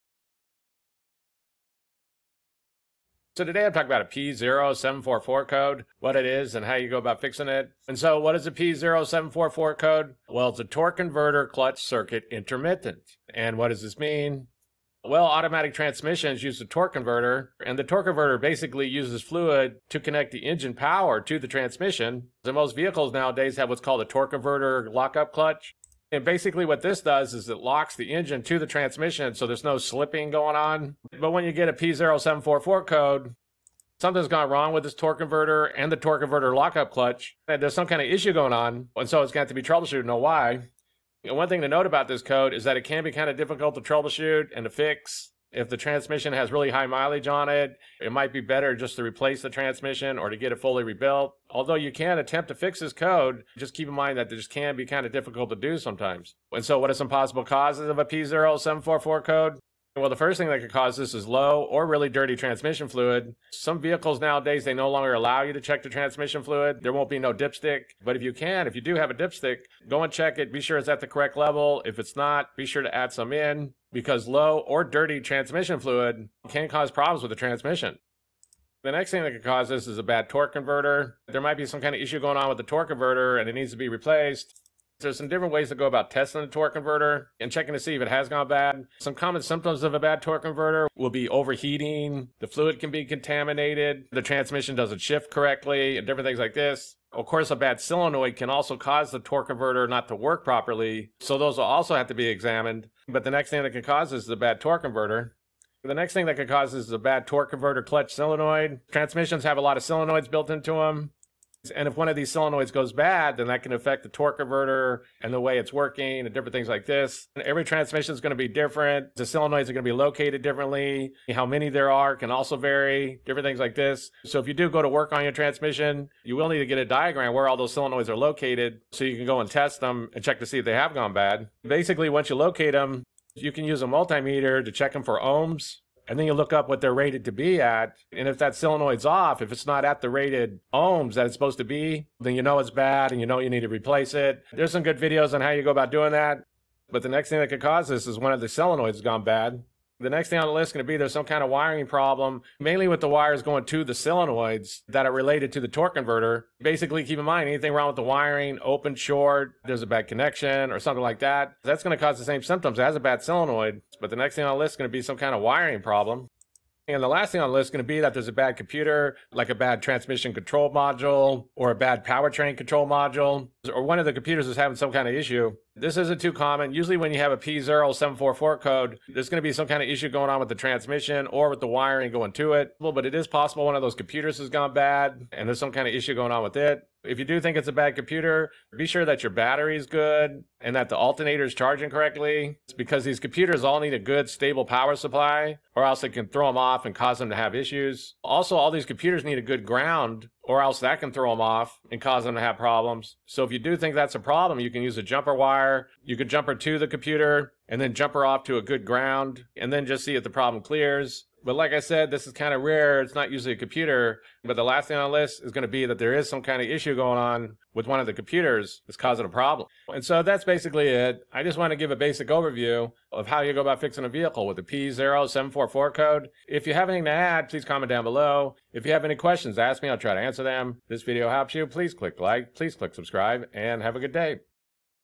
Causes and Fixes P0744 Code: Torque Converter Clutch Circuit Intermittent
How to fix P0744 Code: Torque Converter Clutch Circuit Intermittent
A torque converter is used with automatic transmissions and uses fluid to connect the engine and transmission.
The Torque Converter Lockup Clutch mechanically locks the engine to the transmission so that they rotate at the same speed.
When the computer (TCM/PCM) detects failure of the Torque Converter to lock the engine and transmission together, it will set a P0744 code.
Repairing a transmission with a P0744 Code can be hard to do. If the transmission has high mileage it is often replaced or fully rebuilt.

Possible Causes of a P0744 Code:
Low or Dirty Transmission Fluid
Bad Torque Converter
Bad Torque Converter Clutch Solenoid
Wiring Problem
Bad Computer (TCM/PCM)